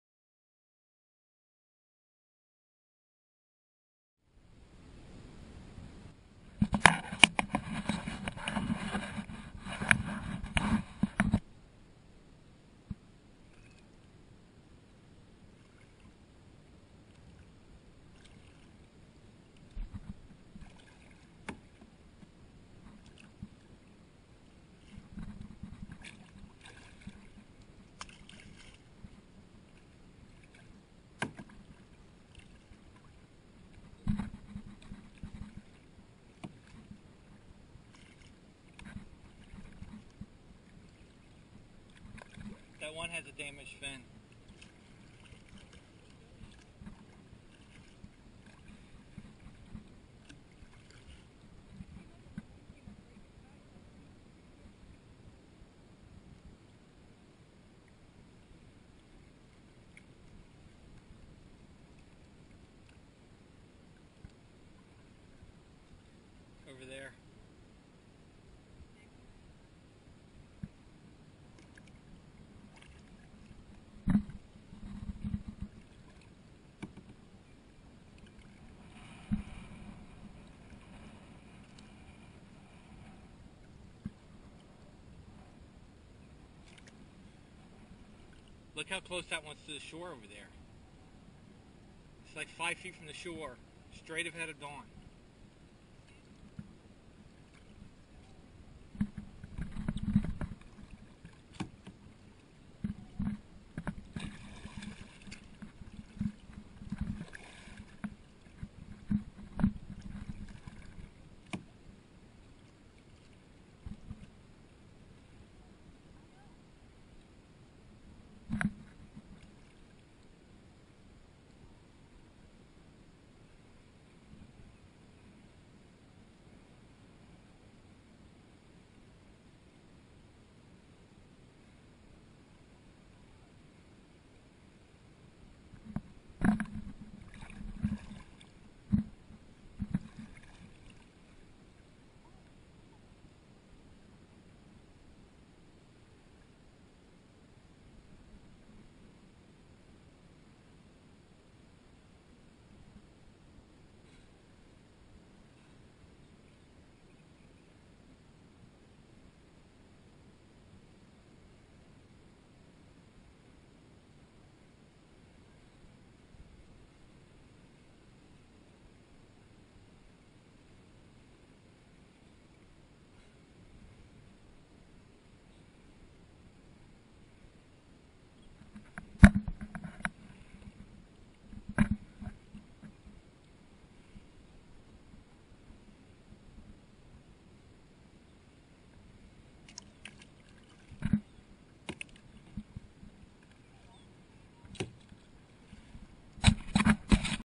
Atlantic Bottlenose Dolphins
Kayaking Intracoastal waterway New Smyrna Beach, FL - Atlantic Bottlenose Dolfins